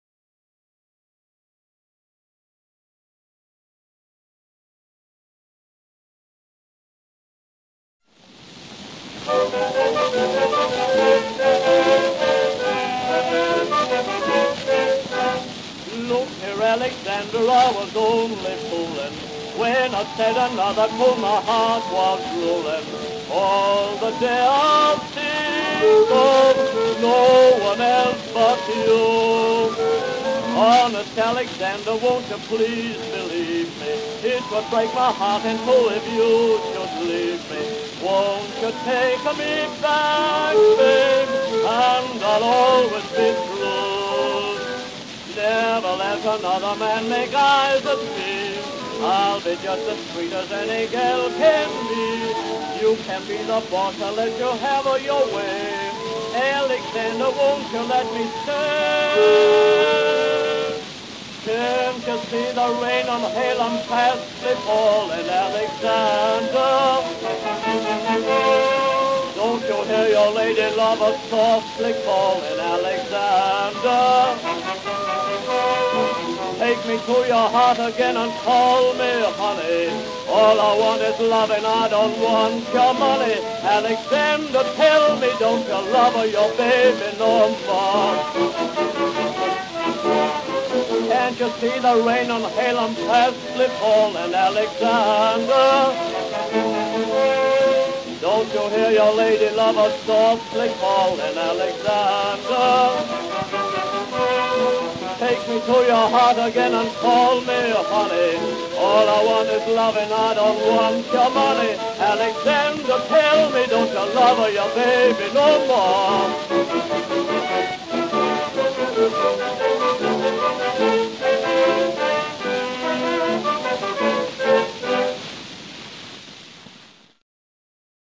nicole.mpg
Nicole Records were the first discs to be wholly produced in the UK. This one features their versatile 'recording expert' and performer Steve Porter. He was from the U.S.A. and could sing in various styles, besides being able to make very good recordings in 1904. The ragtime 'lilt' in this short record is very good. Probably Porter told the band how to play it, not just to read the music? Many later 'ragtime' records made in London don't have this 'lilt'. Three cheers for Steve Porter!